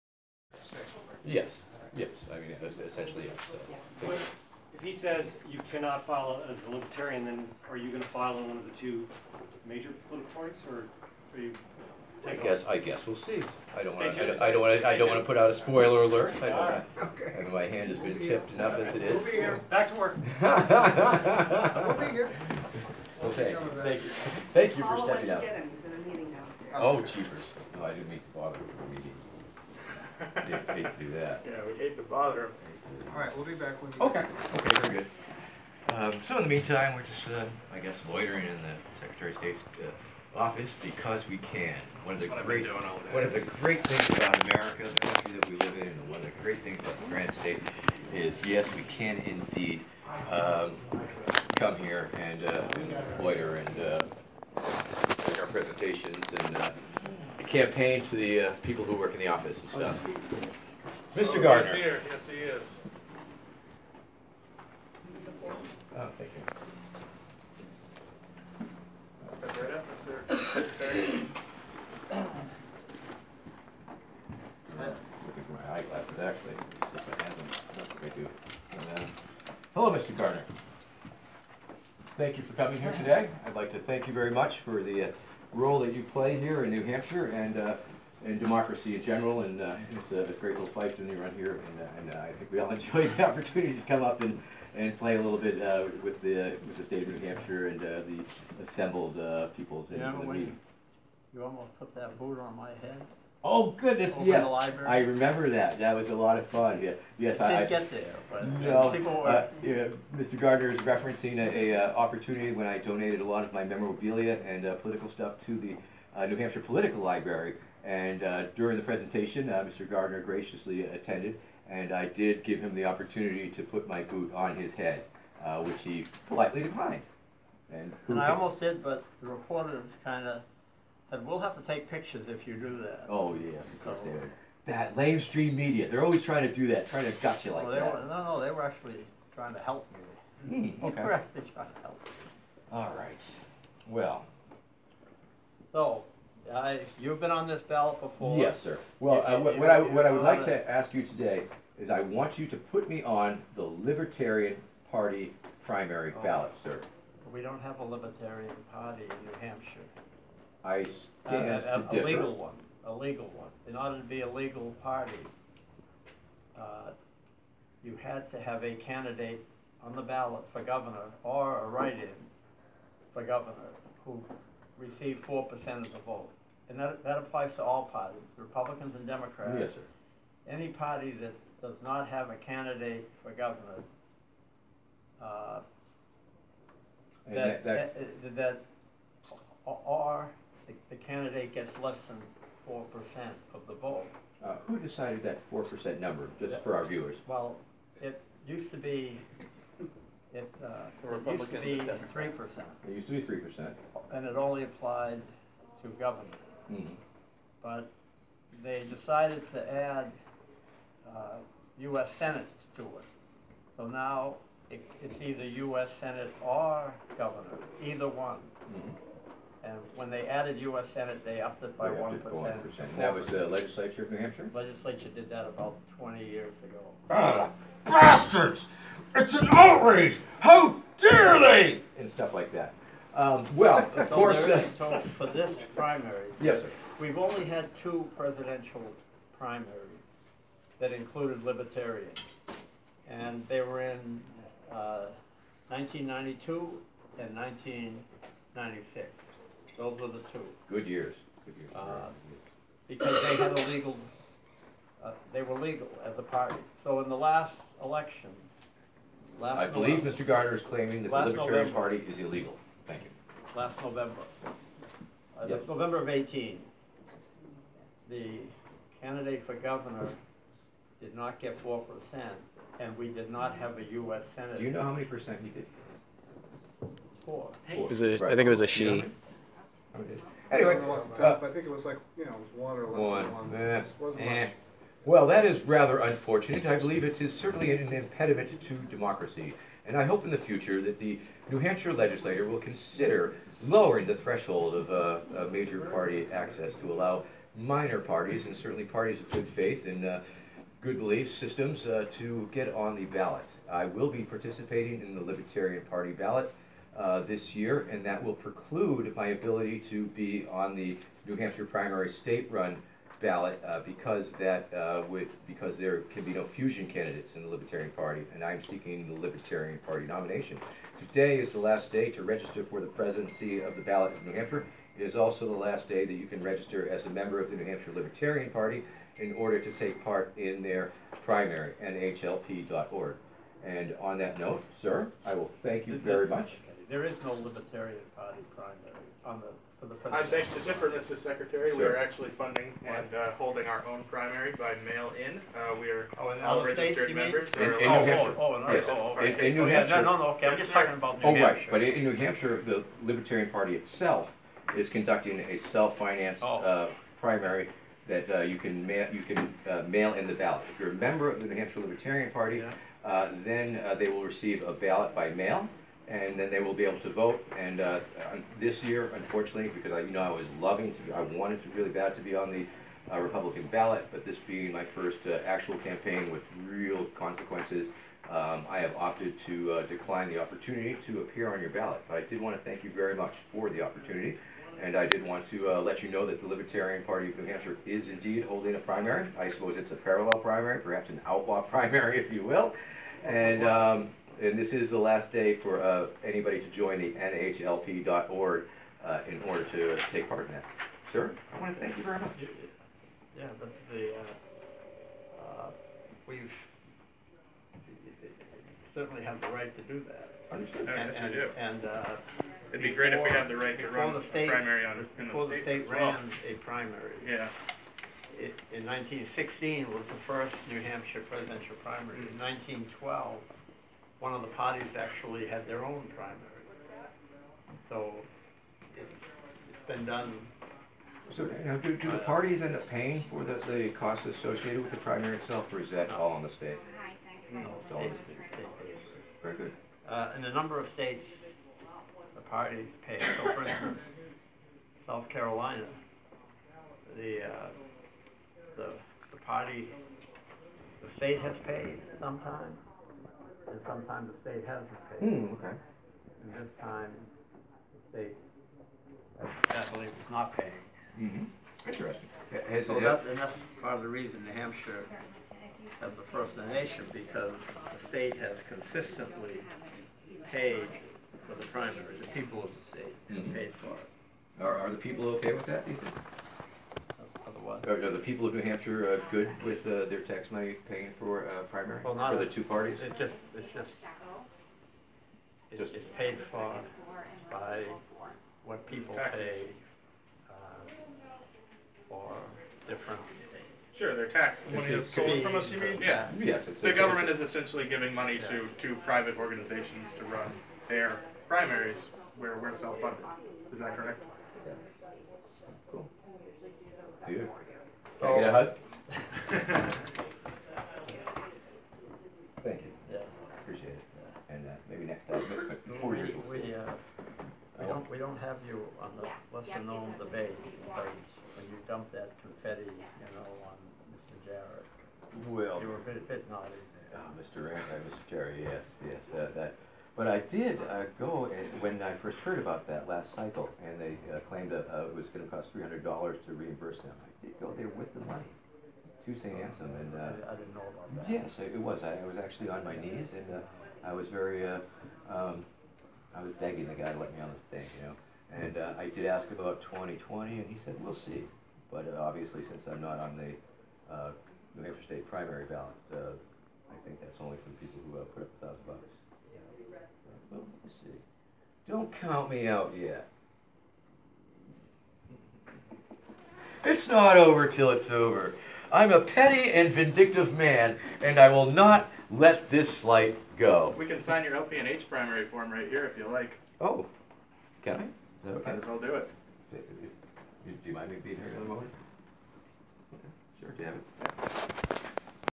Vermin Supreme Registers for NH Presidential Race, 2/3
Vermin Supreme's armed pony procession, a march to the NH state house rallying support for the candidacy of Libertarian nominee hopeful Vermin Supreme, arrives at its destination. Vermin and supporters head inside the NH secretary of state Bill Gardner's office to attempt registry for candidacy in the US presidential race.
Captured using a Samsung Galaxy Tab A